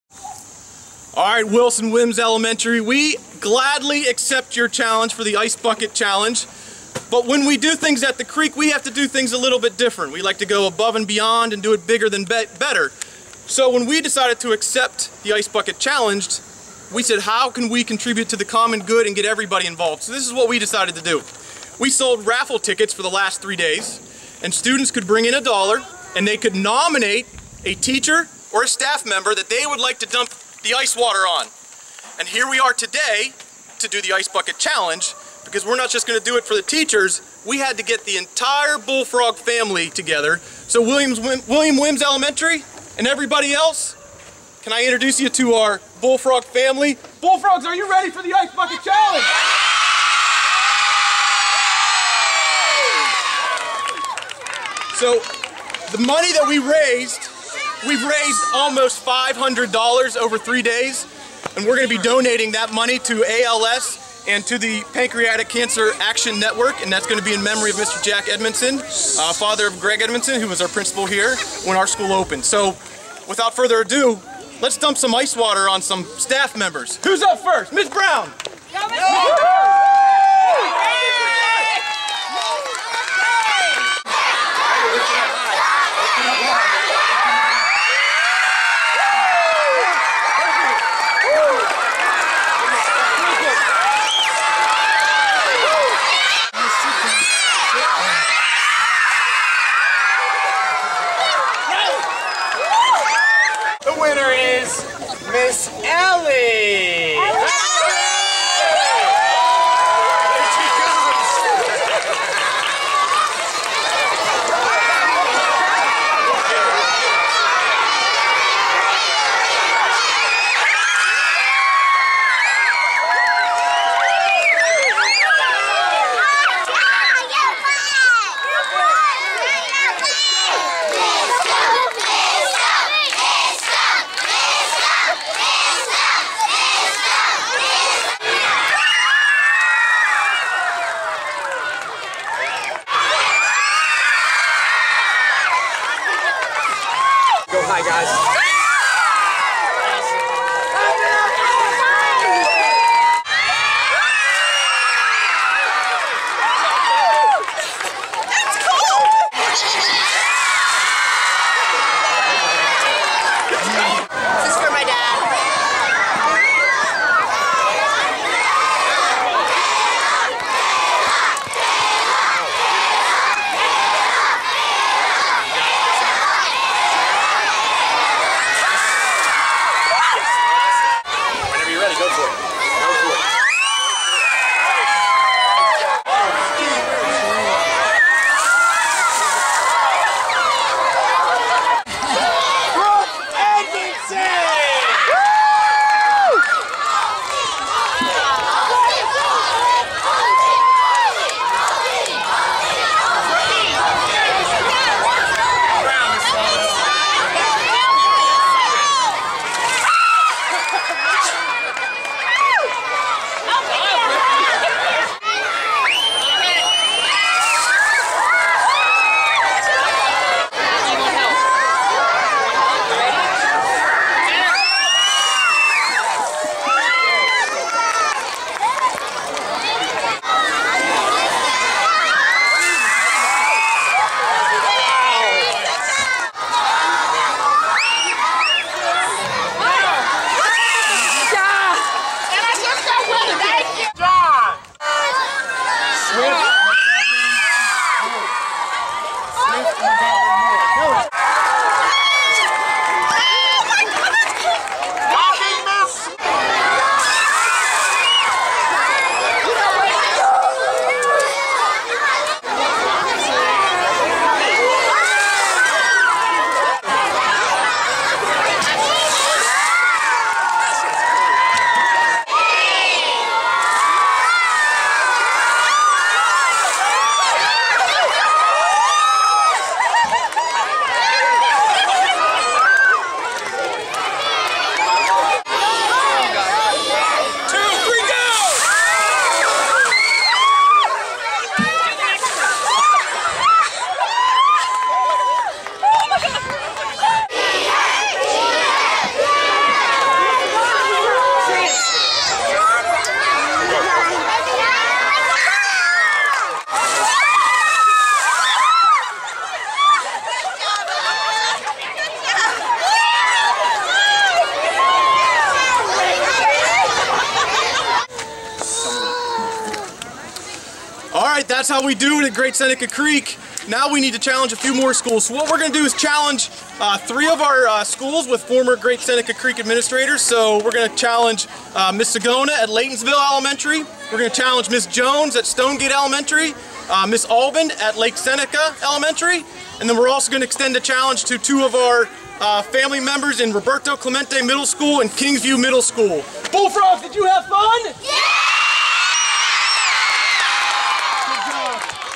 Ice Bucket Challenge Great Seneca Creek Style
In memory of Jack Edmundson (father of Greg Edmundson--founder of GSCES) The Creek Family responded to an Ice Bucket Challenge from Wilson Wims ES to raise money for the Pancreatic Cancer Action Network and the Amyotrophic Lateral Sclerosis (ALS) Association.